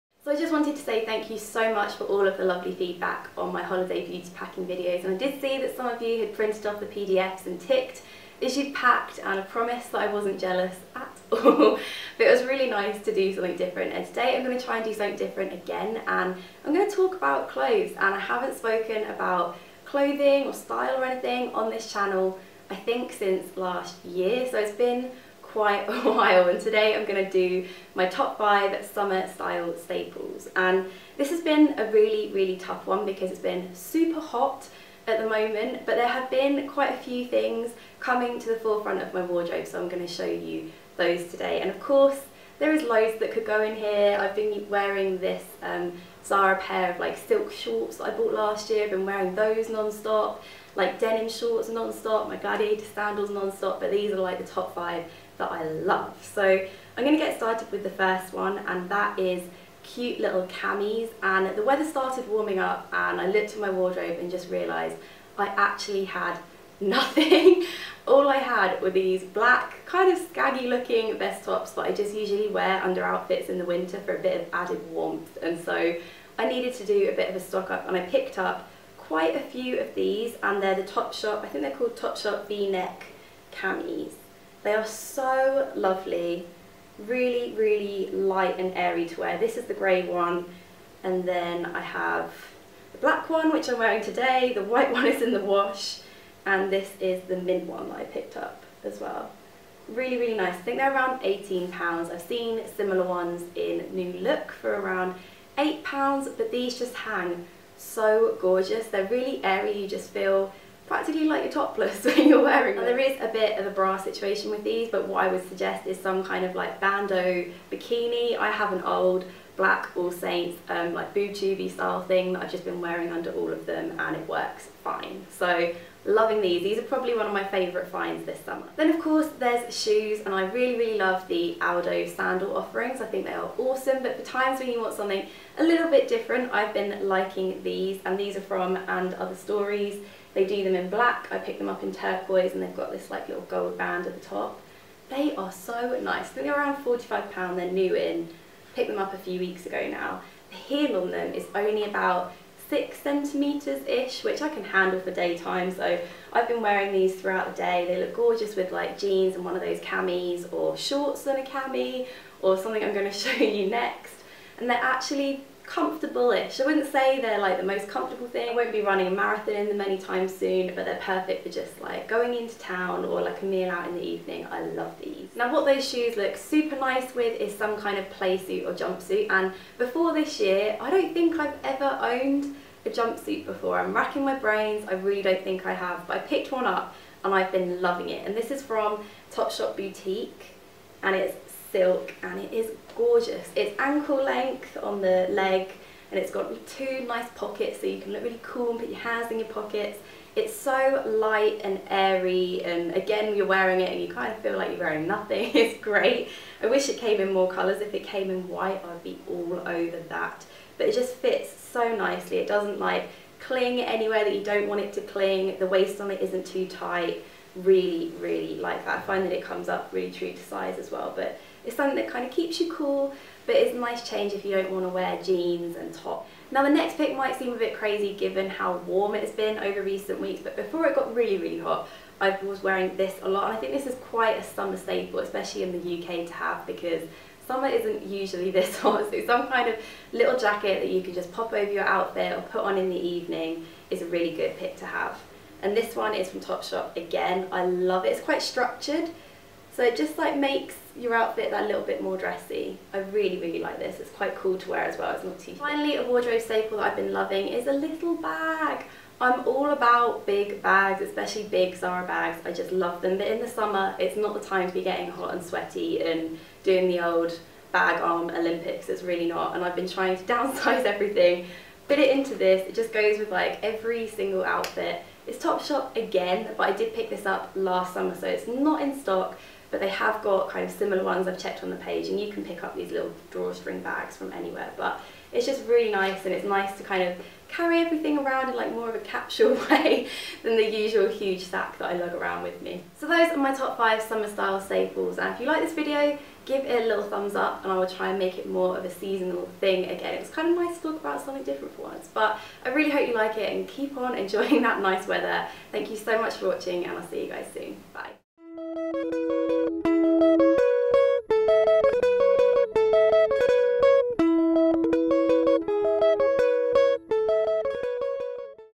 Summer Style Staples | ViviannaDoesMakeup
What I'm Wearing:
Top - Topshop
Watch - Michael Kors 5310
Makeup - Giorgio Armani Luminous Silk Foundation in 6, Bobbi Brown Creamy Concealer in Sand, Kevyn Aucoin The Sculpting Powder in Medium, Hourglass Ambient Lighting Powder in Diffused Light, Hourglass Arch Brow Pencil in Soft Brunette, MaxFactor False Lash Effect Waterproof Mascara, Urban Decay Revolution Lipstick in Streak*
Nails - Bourjois 1 Seconde Nail Enamel in Turquoise Block*
Faux Glow - Caudalie Divine Legs (all over baby)

Topshop Drawstring Bag - not available online, sorry!